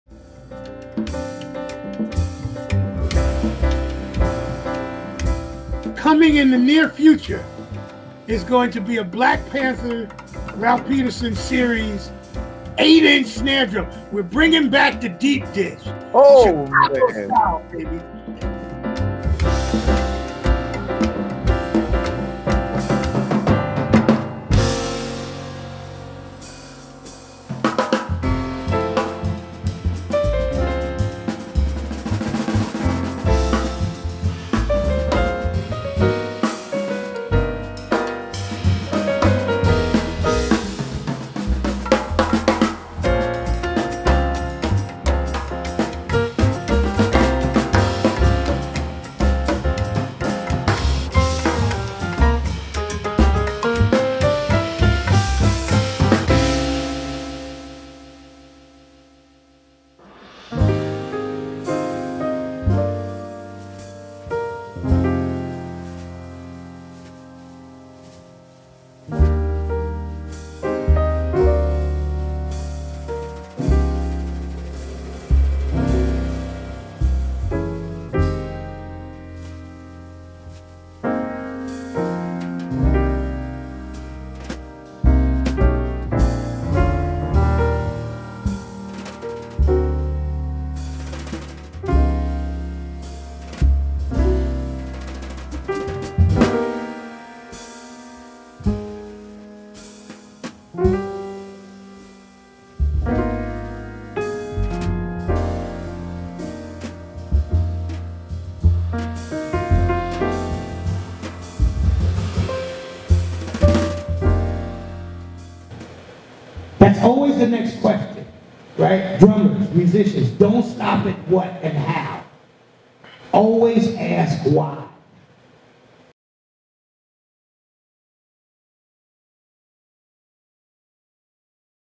The Ralph Peterson Signature Snare Drum
Mapex collaborated with the legendary Ralph Peterson Jr. to craft a beautiful snare that's deep, bright, and aggressive. The instrument is designed to honor his connection to the hard-bop jazz style, with all American maple shells, handcrafted maple hoops, and black plated hardware.